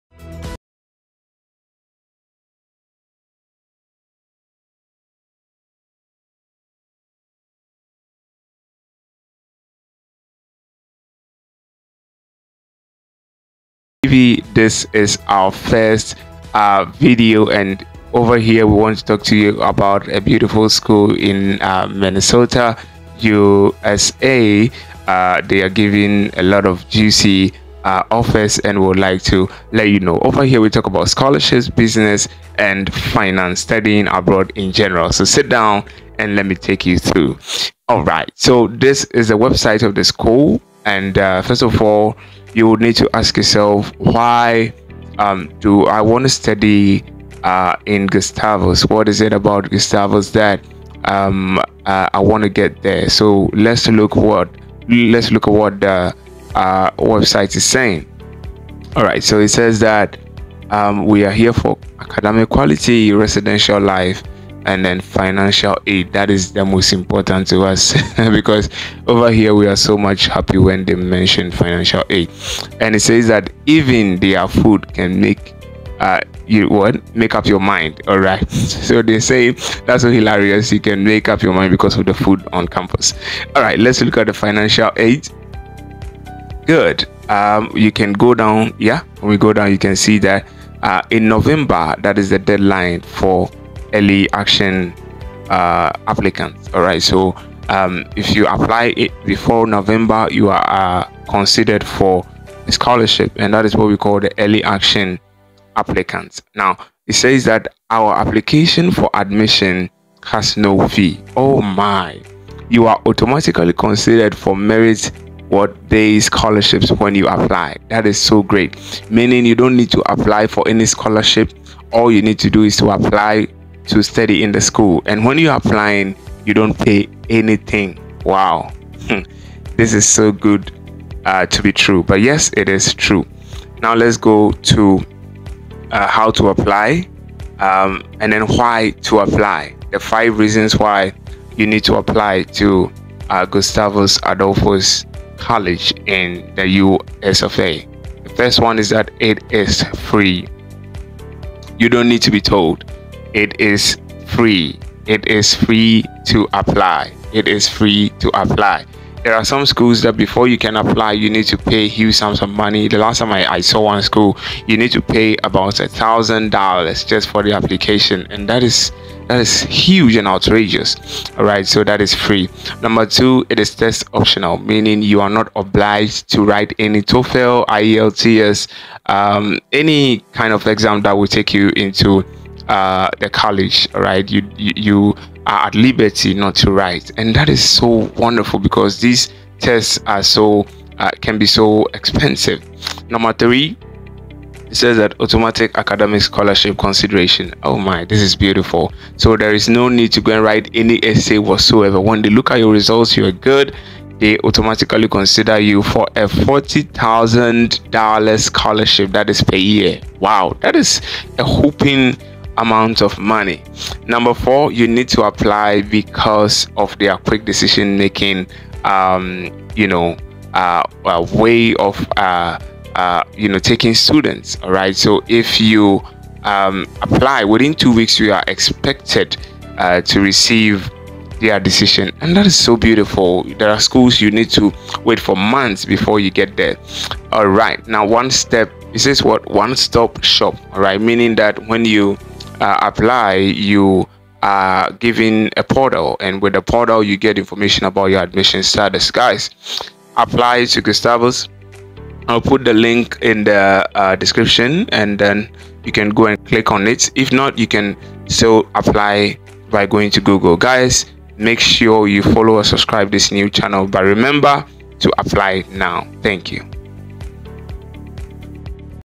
Unlock Your Future:Scholarships with No Applications or Tests Required In This School
Title: Exploring Gustavus Adolphus College: Your Path toExcellence Are you ready to embark on a journey to discover one of the best opportunities for higher education in the United States? Join us today as we unveil the incredible Gustavus Adolphus College, nestled in the picturesque town of St. Peter, Minnesota.
🎓 Unveiling Excellence: Gustavus Adolphus College is renowned for its commitment to academic excellence, fostering a dynamic and inclusive learning environment.

💰 Amazing Scholarships: Here's the exciting news - there's no application fee to apply! Plus, merit students can enjoy an automatic $40,000 entrance scholarship. That's an opportunity worth sharing with aspiring scholars!

🌟 Benefits Beyond Measure: Beyond scholarships, Gustavus Adolphus College offers a world of opportunities, from top-notch faculty to cutting-edge facilities. It's a place where your educational dreams can take flight.

Join us in this eye-opening exploration of Gustavus Adolphus College. Don't miss out on this chance to shape your future and broaden your horizons.

Let's make education accessible and dreams achievable. Together, we can inspire and empower the next generation of scholars.

Don't wait! Dive into the world of possibilities at Gustavus Adolphus College now.